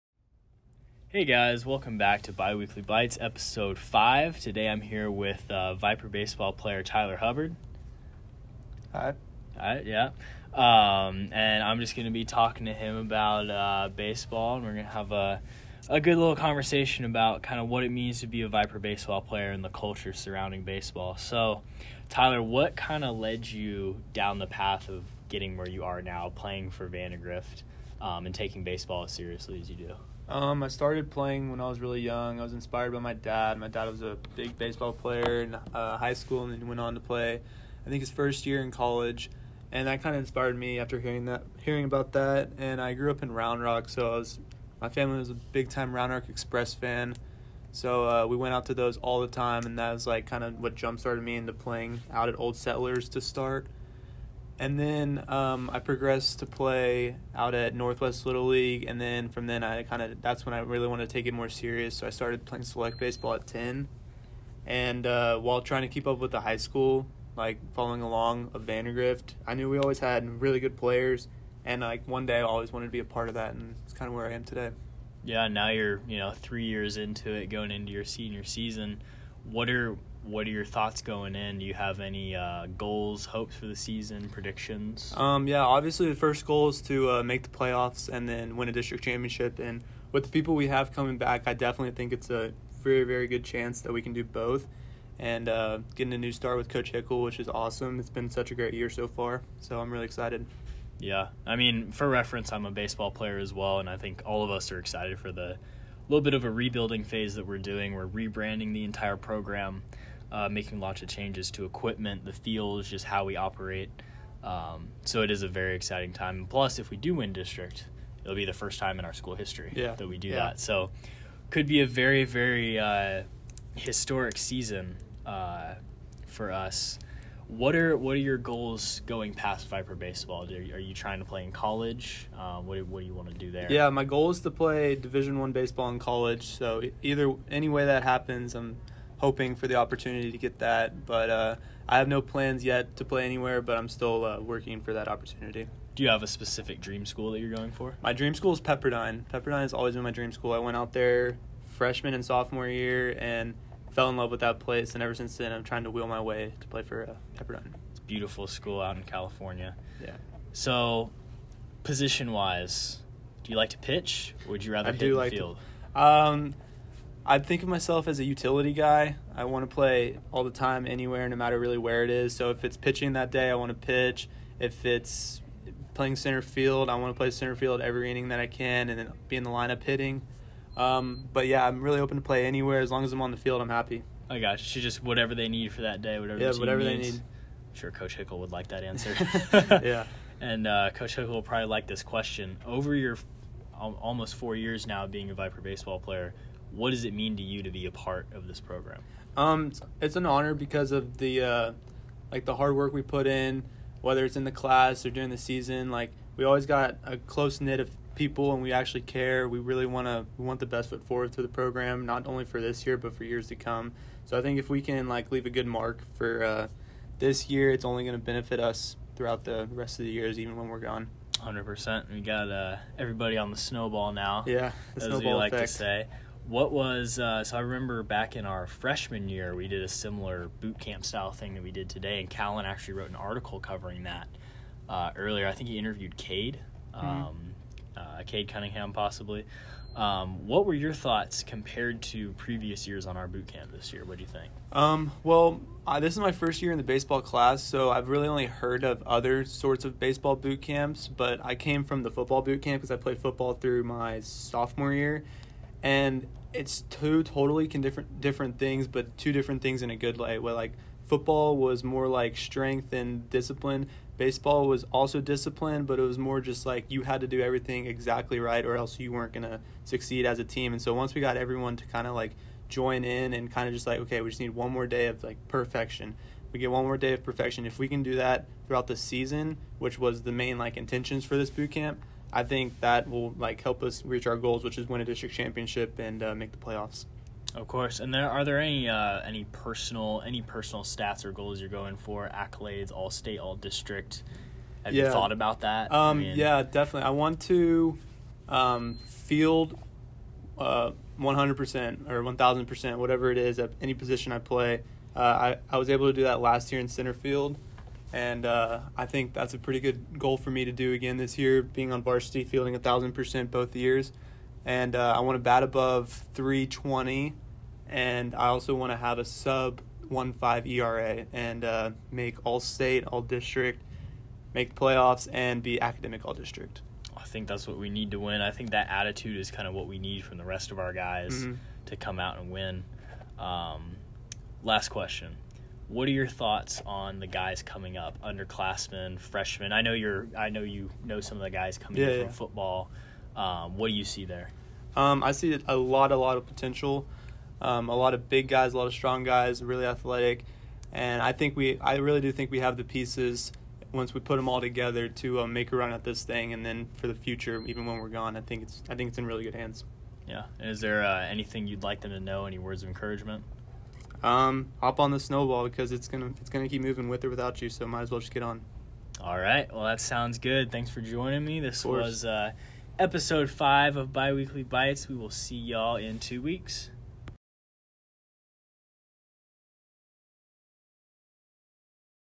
Bi-Weekly Bites Episode 5
Interview with senior baseball player, Tyler Hubbard.